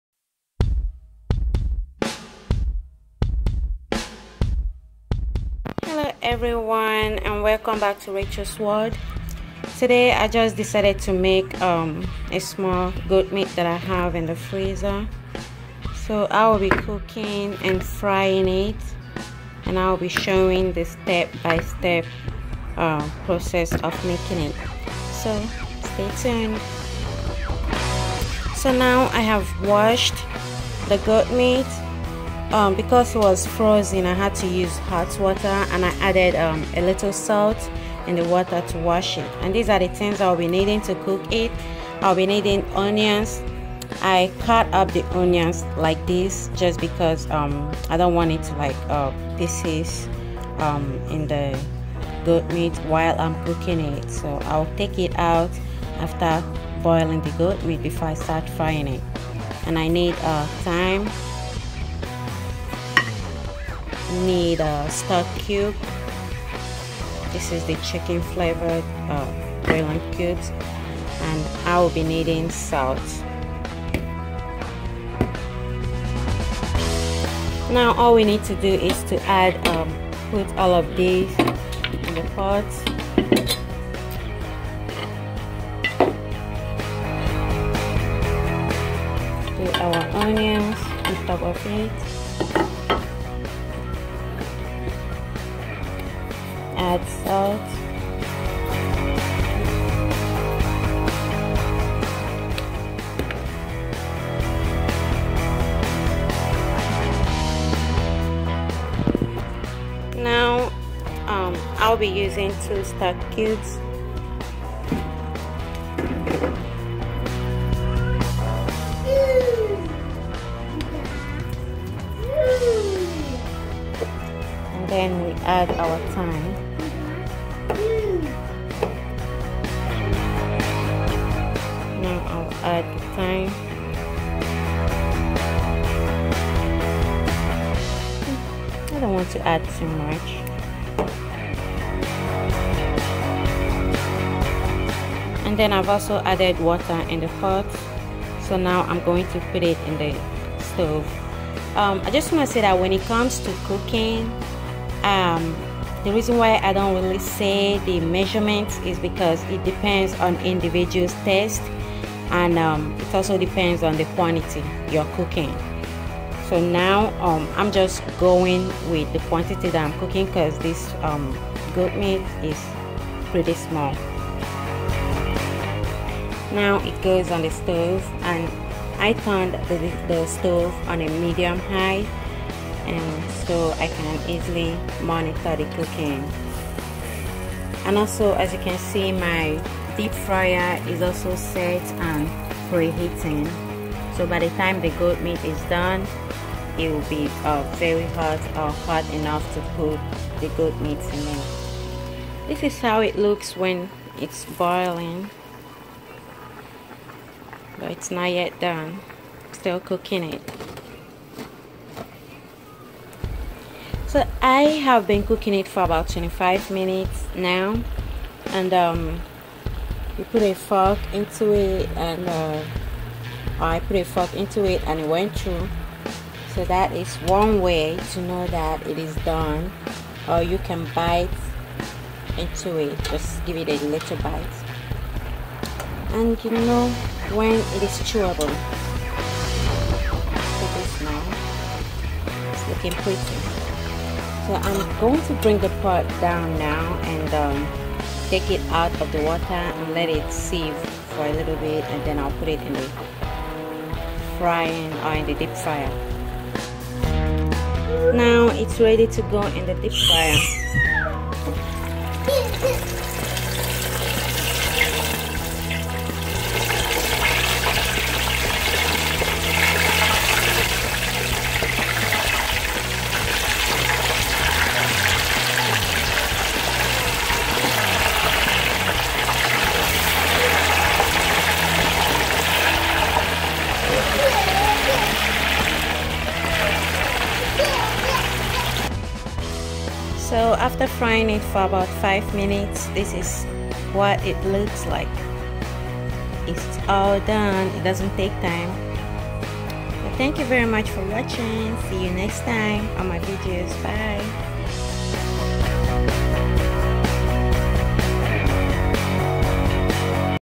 How to cook and fry goat meat
How to cook and fry Nigerian goat meat

Ingredients;
Goat meat
Onions
Thyme
Stuck cube
Salt.

FOLLOW ME//

My blender

My Tripod

Camera 1

Ring light

Studio Light

Camera 2

Mini tripod